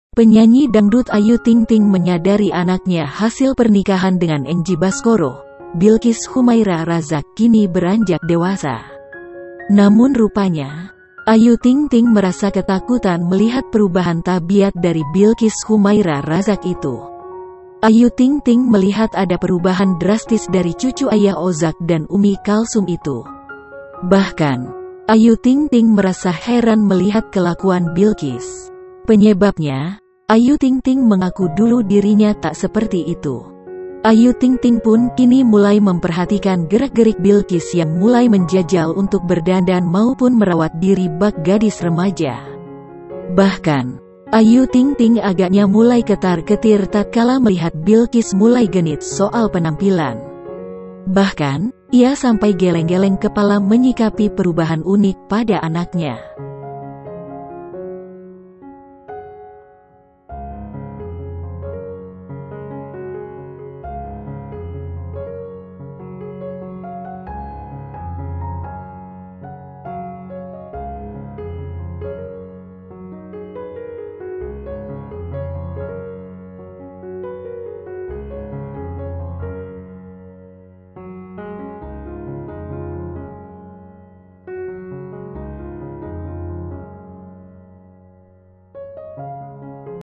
Artis Ayu Ting Ting Akui Takut Lihat Kelakuan Bilqis
TRIBUNLAMPUNG - Penyanyi dangdut Ayu Ting Ting menyadari anaknya hasil pernikahan dengan Enji Baskoro, Bilqis Khumaira Razak kini beranjak dewasa.
Namun rupanya, Ayu Ting Ting merasa ketakutan melihat perubahan tabiat dari Bilqis Khumaira Razak itu.
Ayu Ting Ting melihat ada perubahan drastis dari cucu Ayah Ozak dan Umi Kalsum itu.
Bahkan, Ayu Ting Ting merasa heran melihat kelakuan Bilqis.
Penyebabnya, Ayu Ting Ting mengaku dulu dirinya tak seperti itu.
Ayu Ting Ting memang sudah menjadi orang tua tunggal bagi putrinya, Bilqis Kumairah Razak, sejak hamil.
Oleh karena itu, Ayu Ting Ting tak segan menumpahkan rasa kasih sayangnya kepada sang anak semata wayang.
Ayu Ting Ting rupanya sadar betul jika sang buah hati sudah tak mendapatkan kasih sayang dari seorang ayah.
Bahkan sejak Bilqis masih dalam kandungan Ayu Ting Ting.
Ya, melansir dari Grid.ID, sudah bukan rahasia lagi, Ayu Ting Ting memang sudah berpisah dari mantan suami, Enji Baskoro sejak dirinya hamil besar.
Tak sampai di situ saja, Bilqis Khumairah Razak pun disebut-sebut belum pernah bertemu lagi dengan ayahnya sampai saat ini.
Sejak perpisahannya dengan Enji Baskoro, Ayu Ting Ting tak mau bila anak yang dilahirkannya kekurangan kasih sayang.
Biduan dangdut asal Depok ini pun memang sangat memperhatikan tumbuh kembang putri semata wayangnya tersebut.
Karena hal itu juga, Ayu Ting Ting mengaku sangat merasakan bagaimana tumbuh kembang sang buah hati dri waktu ke waktu.
Pada 28 Desember 2021 lalu, tepat di hari ulang tahunnya, perayaan kecil-kecilan dilakukan oleh Ayu Ting Ting untuk Bilqis di Pulau Bali.
Kini Bilqis Khumairah Razak diketahui telah memasuki usia delapan tahun.
Dengan kata lain, putri Ayu Ting Ting tersebut sudah beranjak menuju seorang anak baru gede (ABG).
Ternyata pertumbuhan Bilqis yang beranjak ABG tersebut juga menjadi perhatian khusus bagi Ayu Ting Ting.
Bagaimana tidak? Ayu Ting Ting sampai dibuat ketar-ketir dengan pertumbuhan sang putri yang menjadi sorotannya saat ini.
Ayu Ting Ting pun kini mulai memperhatikan gerak-gerik Bilqis yang mulai menjajal untuk berdandan maupun merawat diri bak gadis remaja.
Bahkan, Ayu Ting Ting agaknya mulai ketar-ketir tatkala melihat Bilqis mulai genit soal penampilan.
Ayu Ting Ting mengungkap perubahan dalam diri Bilqis saat disambangi awak media, seperti dikutip dari tayangan di kanal YouTube Surya Citra Televisi (SCTV), Jumat (7/1/2022).
Awalnya Ayu Ting Ting berharap di tahun 2022 ini menjadi tahun yang baik untuk dirinya.
"Sehat terutama, biar lebih sukses karier saya ke depannya, terus biar lebih baiklah."
"Menjadi pribadi yang jauh lebih baik, biar semakin banyak program, aamiiin," ucap Ayu Ting Ting.
Lebih lanjut, Ayu Ting Ting pun dicecar pertanyaan seputar Bilqis yang belum lama ini merayakan hari ulang tahunnya.
Penasaran dengan doa dan harapan untuk Bilqis, Ayu Ting Ting pun tak sungkan membeberkan banyak hal.
Tak hanya doa, namun Ayu Ayu Ting Ting juga mengaku ketar-ketri dengan perubahan sang buah hati.
Bahkan, ia sampai geleng-geleng kepala menyikapi perubahan unik pada anaknya.
"Makin cerewet, makin bawel, makin pinter, udah bisa protes, udah bisa ngomelin orang, wah udah macem-macem deh," kata Ayu Ting Ting.
"Apalagi perempuan kan, makin genit, kerjaannya pake masker wajah terus."
"Masker anak-anak itu beli waduh banyak banget, ada kali satu boks dia beli."
"Tiap malem maskeran, padahal dulu saya (kecilnya) enggak pernah begitu," pungkas Ayu Ting Ting, melansir Sosok.ID.